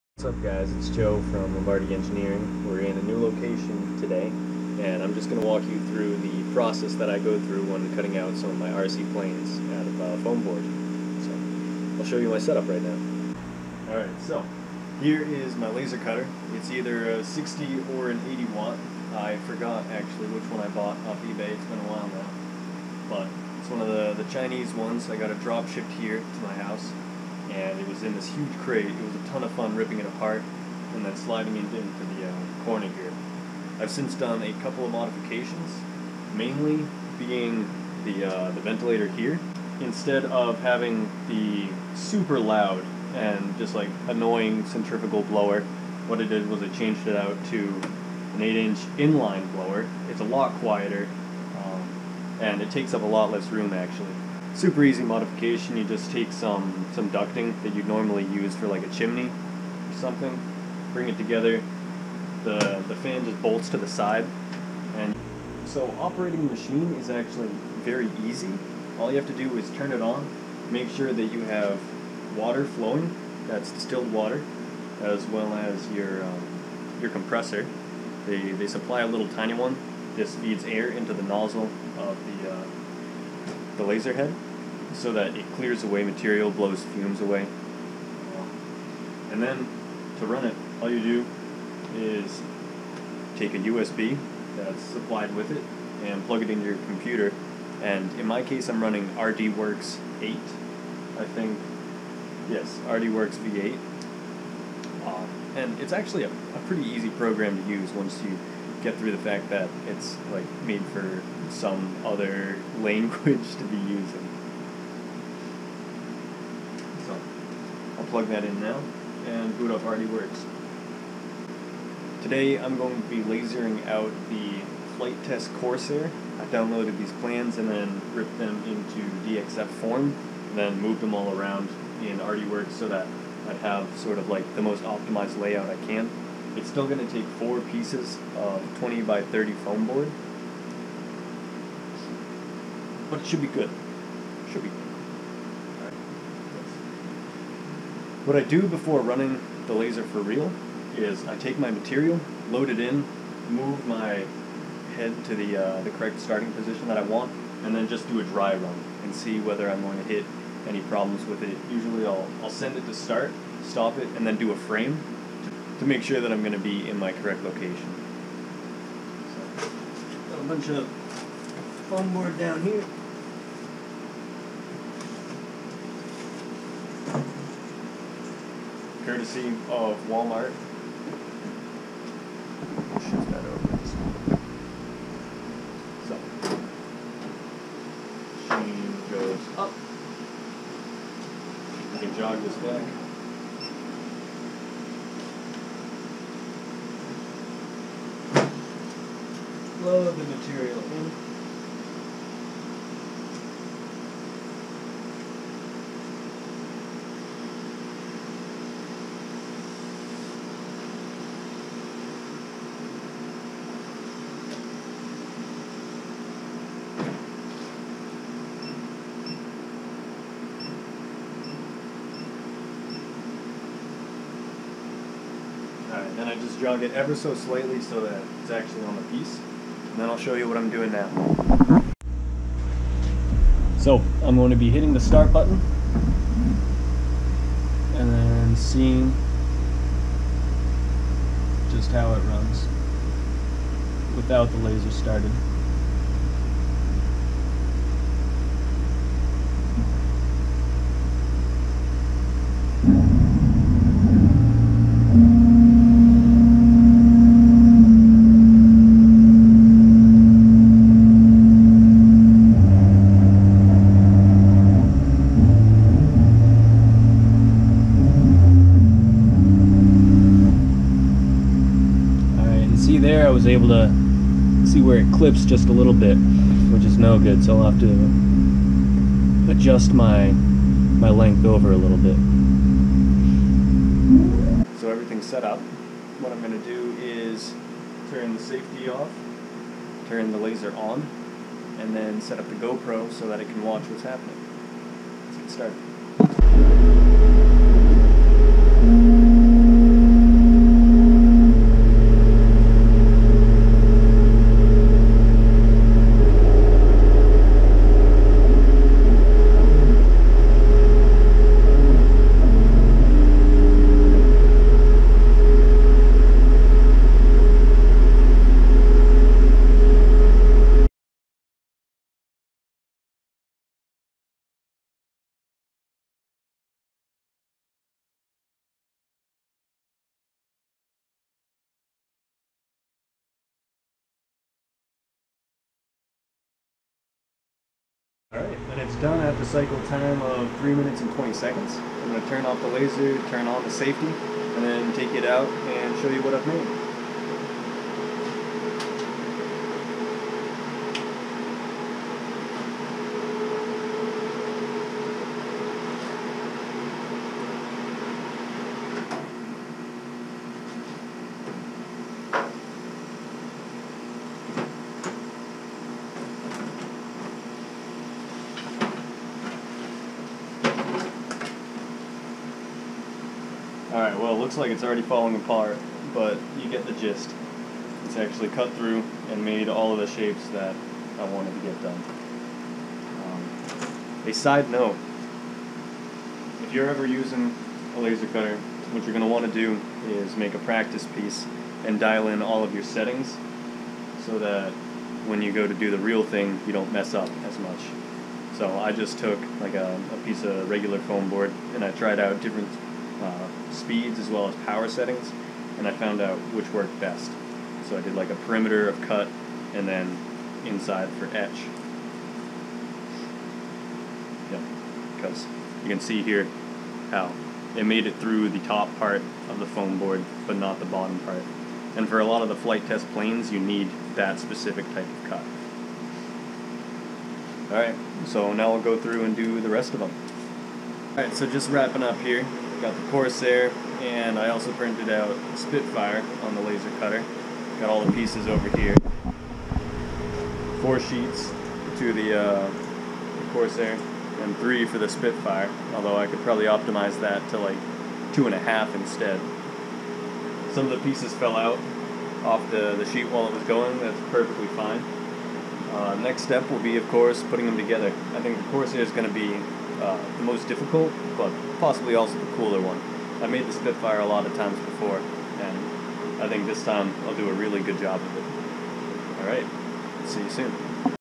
Chinese Laser Cutter (Cutting FliteTest Airplanes)
An intro to how I cut out Flite test airplanes. You can use the dxf files from here: